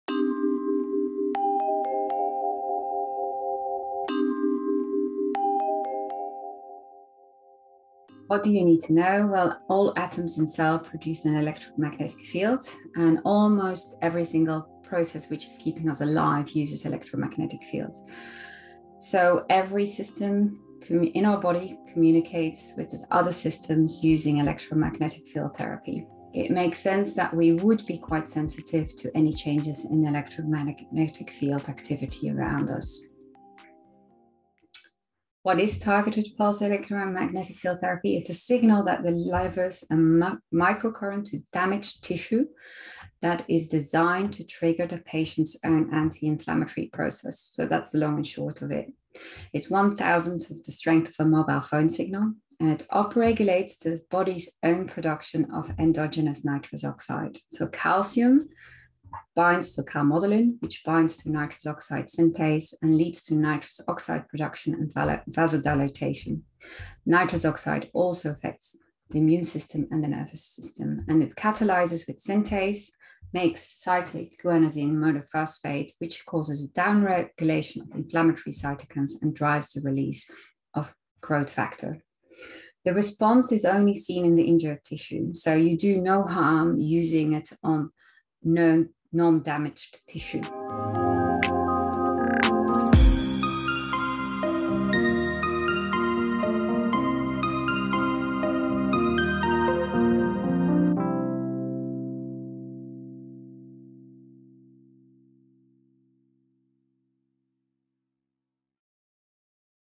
What is PEMF?
Dr Veerle Dejonckheere talks PEMF at The 2021 Munch & Learn, Companion Animal CPD.

Veerle qualified as a veterinary surgeon from the University of Gent, Belgium, in 2001. Moving to the UK in 2003 to work in small animal general practices across the country.

Veerle has always been interested in combining conventional and complementary medicine to maximise the health of her patients. She specialised in rehabilitation and canine sports medicine in 2009 and trained in Veterinary Chiropractic, qualifying in April 2014.